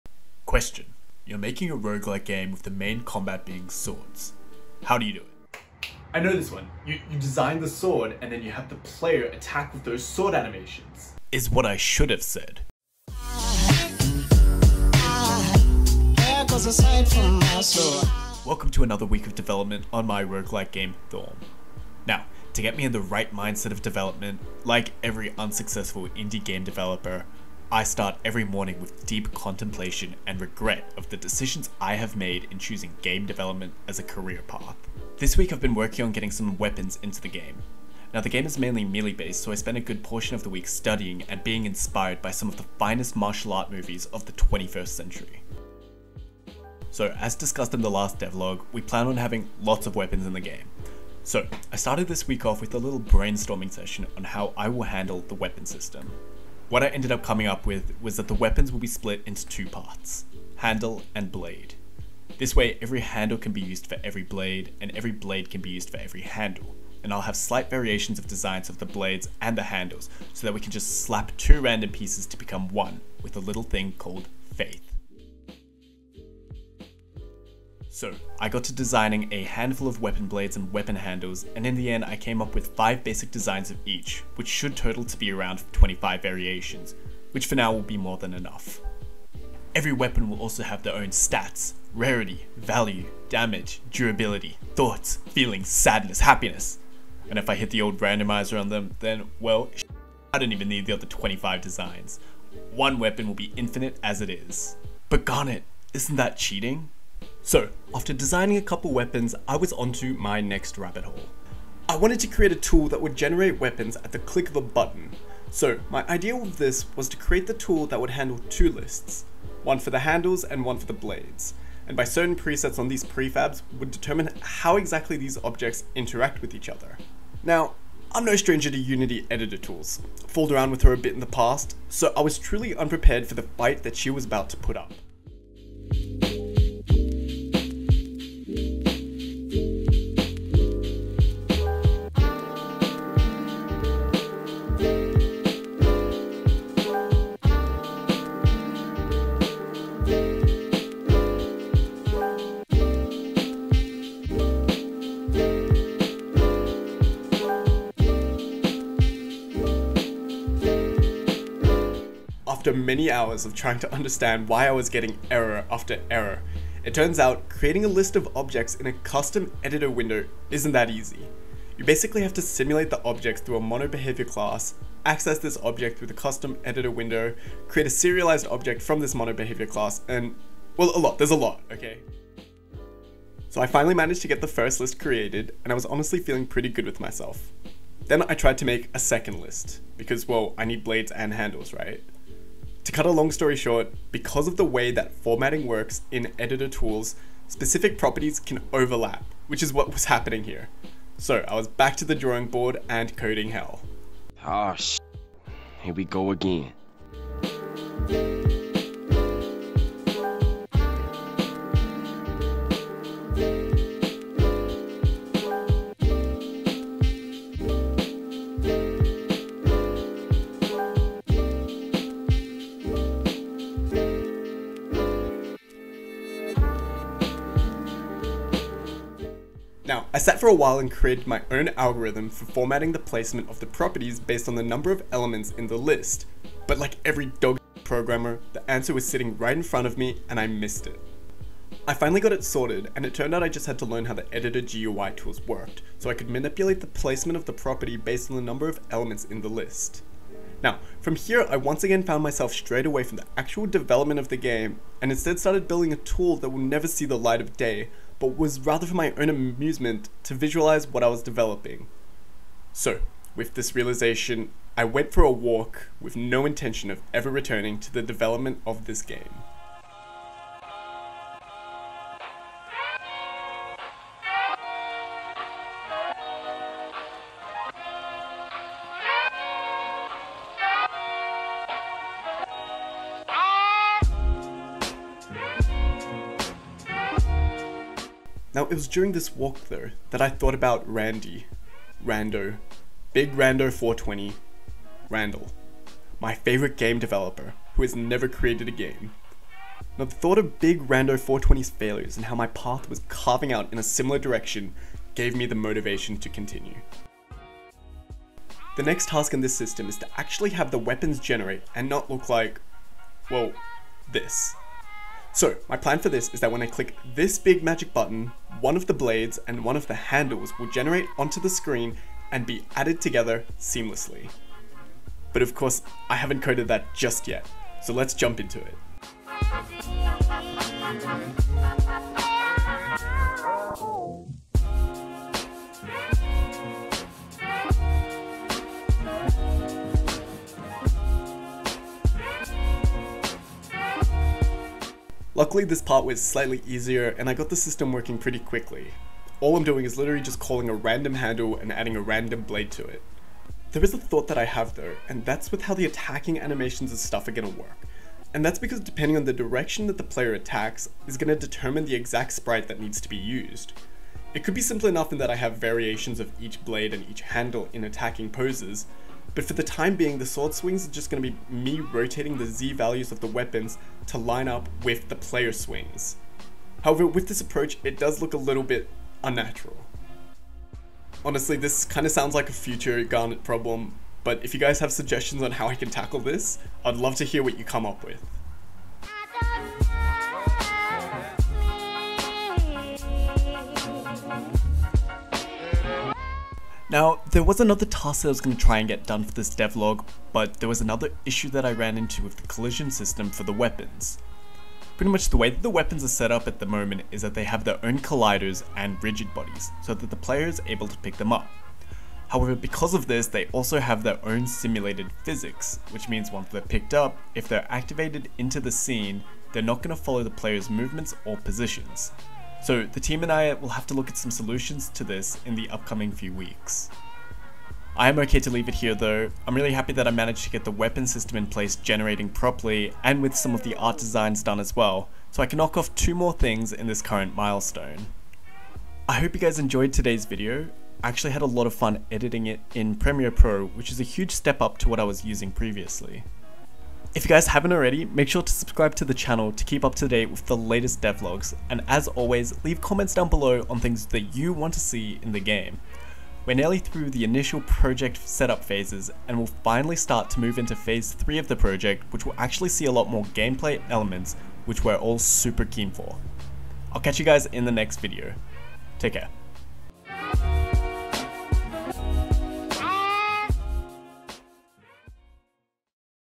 making weapons for my dream game!! | devlog 2
In today's video, I create a tool that will allow our upcoming roguelike to have infinite weapon designs that can be used by both the players AND the enemies.

My Games:
Credits:
Music used in video:
Chapters
0:00 Intro
0:18 Existential Crisis
0:46 Designing Weapons
1:47 Weapon Generator Tool
4:23 Existential Crisis 2
5:08 Procedural Weapon System
6:36 Plans for the future
7:17 Outro